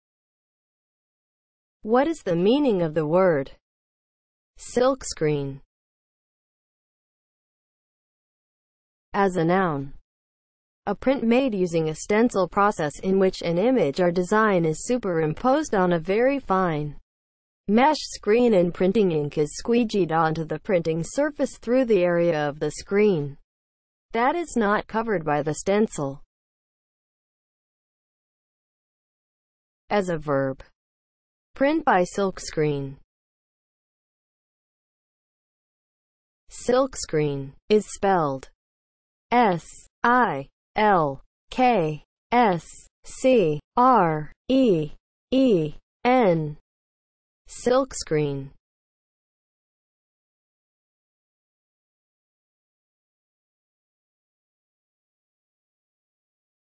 What is the meaning of the word SILKSCREEN?
Definition and spelling of the word SILKSCREEN.

(00:00) Intro
(00:08) As a noun
(00:28) As a verb
(00:34) Spelling

As a noun (00:08):
• a print made using a stencil process in which an image or design is superimposed on a very
  fine mesh screen and printing ink is squeegeed onto the printing surface through the area of
  the screen that is not covered by the stencil

As a verb (00:28):
• print by silkscreen